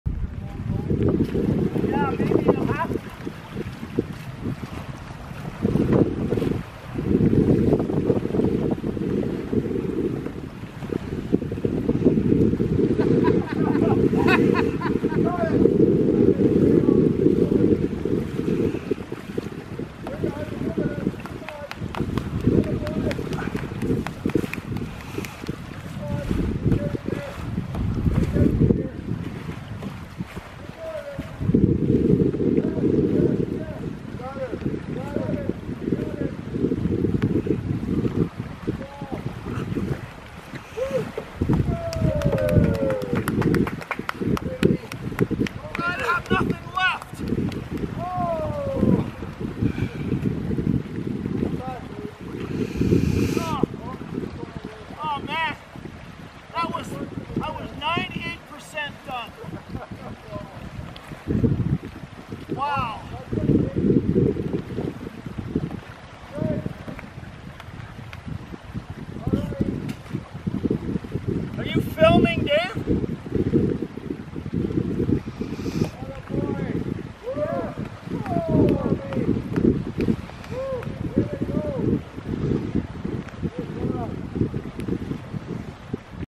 Ron and Lee kayaking at Winter Cove, Saturna Island
Two intrepid kayakers play in the tidal rapids.